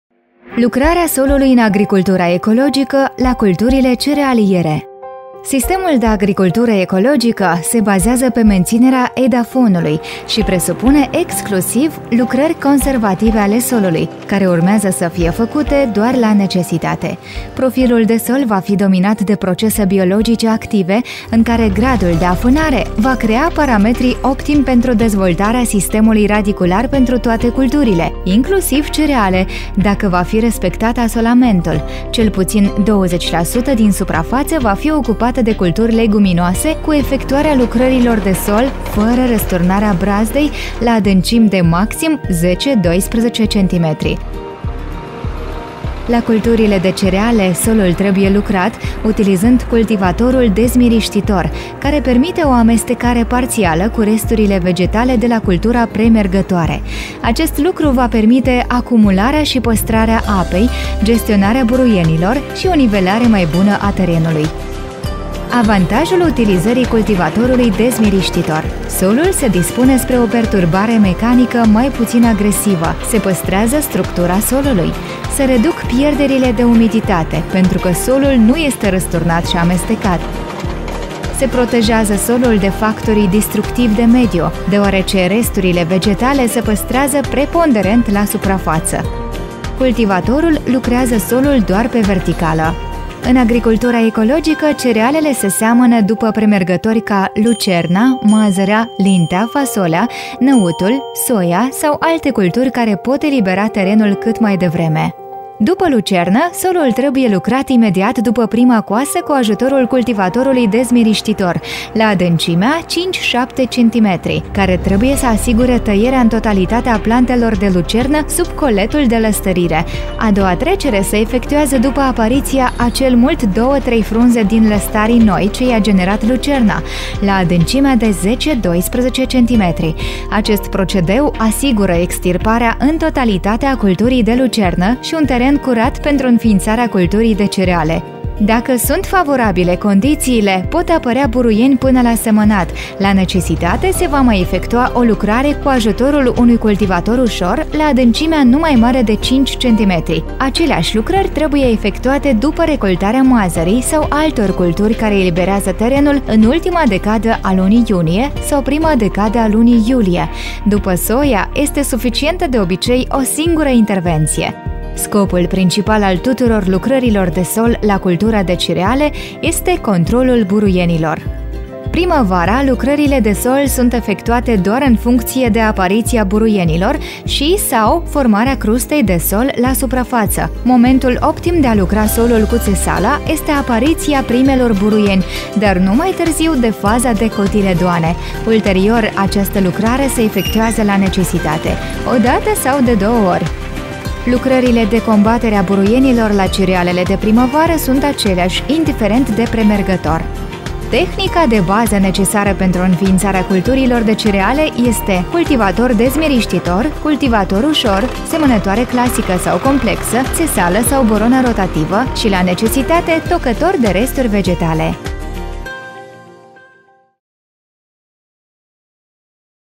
Lucrarea solului în agricultura ecologică la culturile cerealiere | InfOrganic
Sistemul de agricultură ecologică se bazează pe menținerea edafonului și presupune exclusiv lucrări conservative ale solului, care urmează să fie făcute doar la necesitate.

De asemenea, mai multe informații le puteți găsi în următorul infografic

Materialul video a fost realizat în cadrul proiectului ”InfOrganic Moldova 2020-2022”, implementat de AED, cu suportul financiar al Fundației LED. AED mulțumește EcoVisio, pentru produsul elaborat!